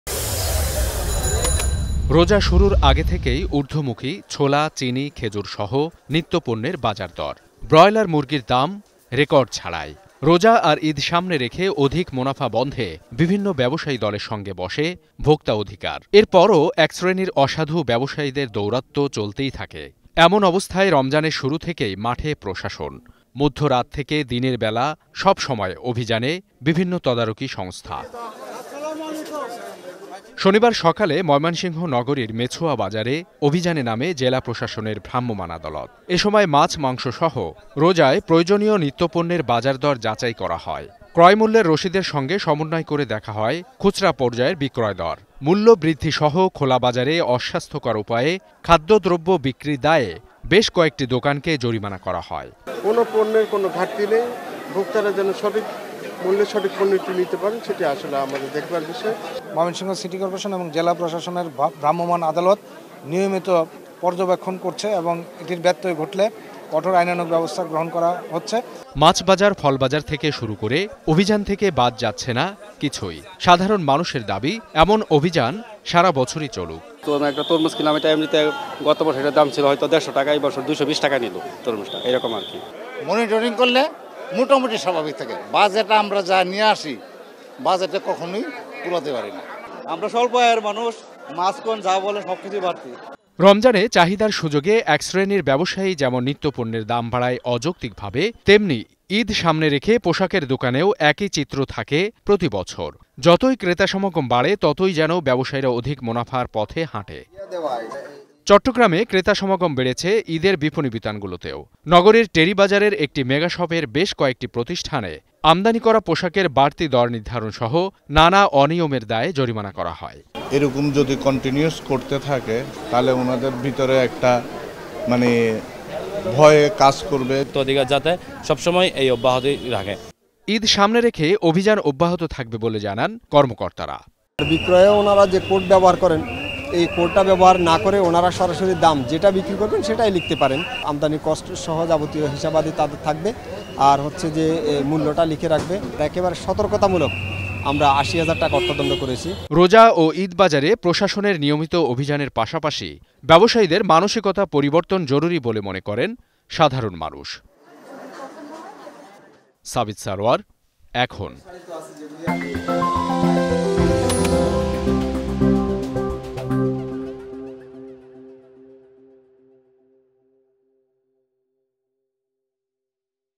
ব্যবসায়ীদের কারসাজি বন্ধে তৎপর প্রশাসন | Vokta Odhikar | Ekhon TV
রোজা ও ঈদের বাজারে অসাধু ব্যবসায়ীদের কারসাজি বন্ধে সারাদেশে অভিযানে নেমেছে ভোক্তা অধিকার ও স্থানীয় প্রশাসন। মাছ, মাংস ও ভোগ্যপণ্যের পাশাপাশি বিপণি বিতানগুলোতে চলছে অভিযান। এ সময় ক্রয় মূল্যের রশিদের সঙ্গে খুচরাপর্যায়ে বিক্রয় মূল্যের তালিকা সমন্বয় করে বেশকিছু অনিয়ম ধরা পড়ে। বাড়তি দরসহ নানা অনিয়মের দায়ে প্রতিষ্ঠানগুলোকে জরিমানা ও সতর্ক করা হয়েছে।

Our Office Address:
EKHON TV
City Park Lane
19, Hatkhola Road, Wari
Dhaka-1203
Bangladesh